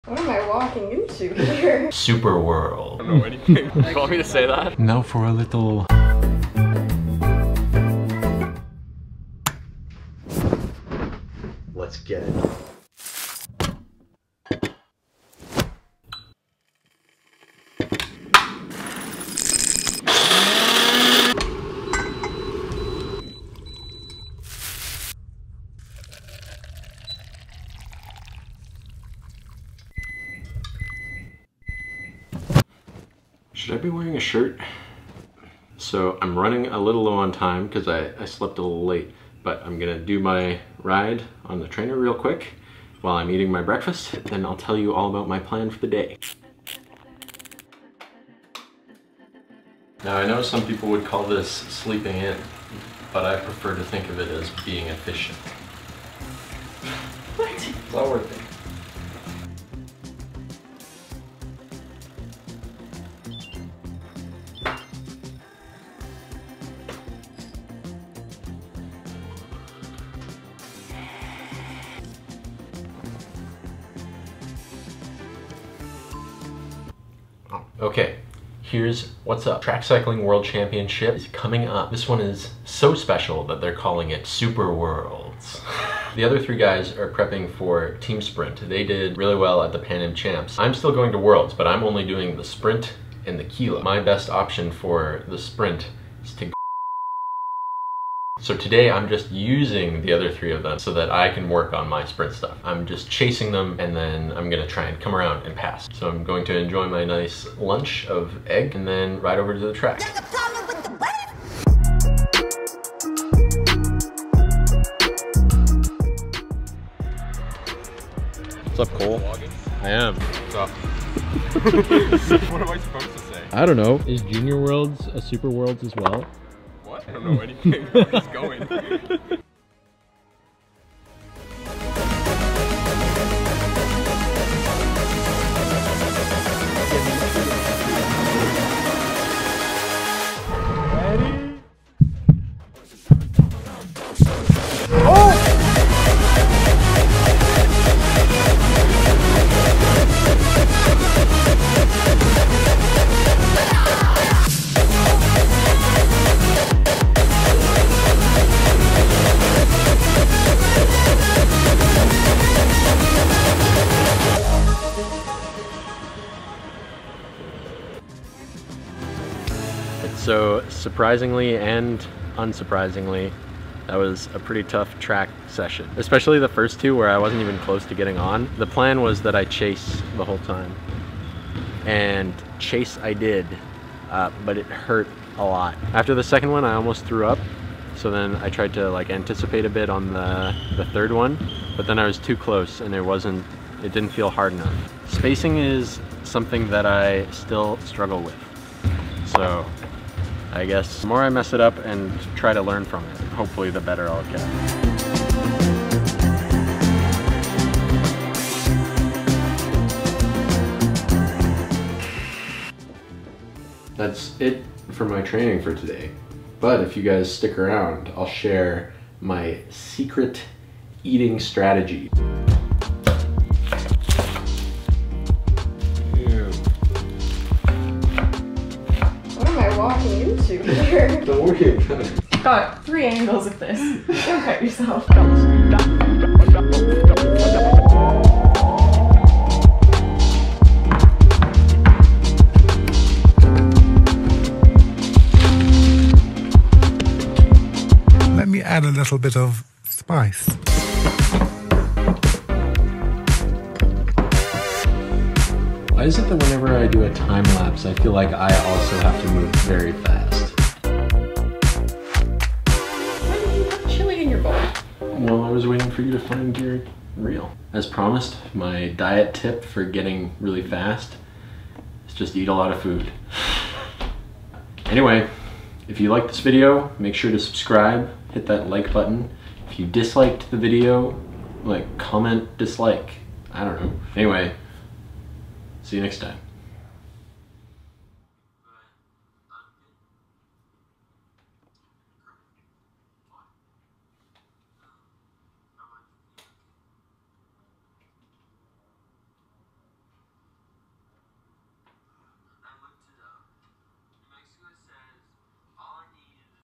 UCI World Championship Sprint Prep | Day in the Life
A full day in my life, along with some pro tips!

Web: dodyk.ca